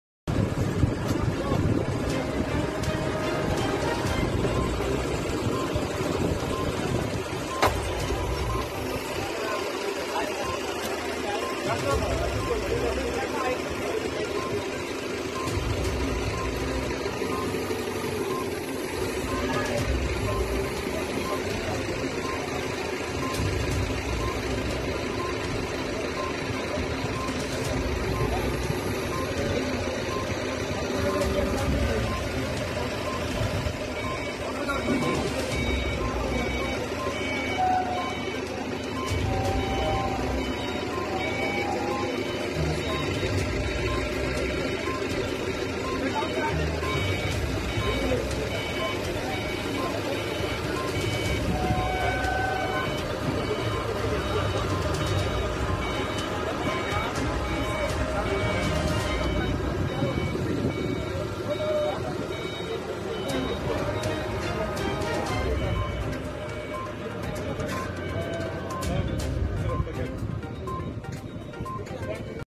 AFGHANISTAN: Pakistan's Humanitarian Aid Arrives in Kabul Five Tonnes of Flour, Oil and Medicine.
Pakistan's humanitarian aid arrives in Kabul Five tonnes of flour, oil and medicine donated by Pakistan to Afghanistan arrived at Hamid Karzai International Airport